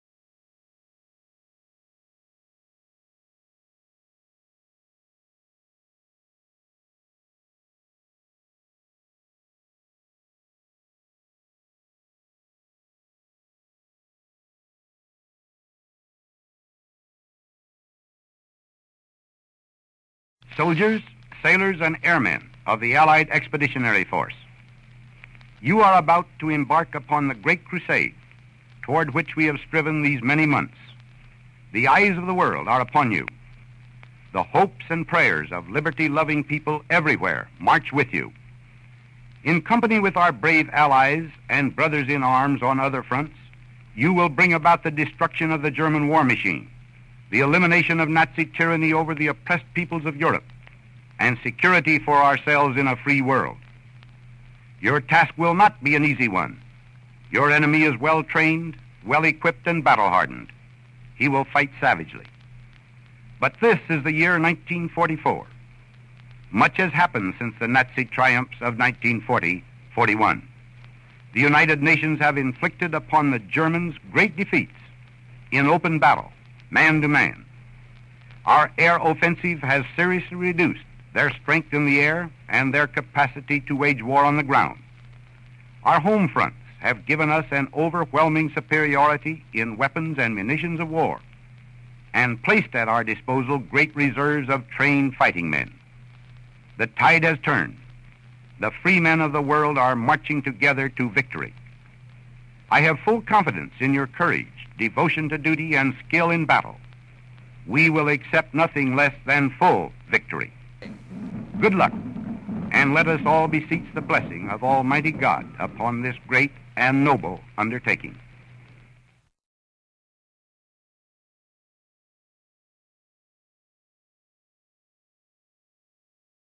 US Army 14th Major Port Transportation Corps receive the Freedom of Southampton 1946
Southampton's Civic Centre was the headquarters for the US Army 14th Major Port and the de facto base for D-Day and Operation Overlord.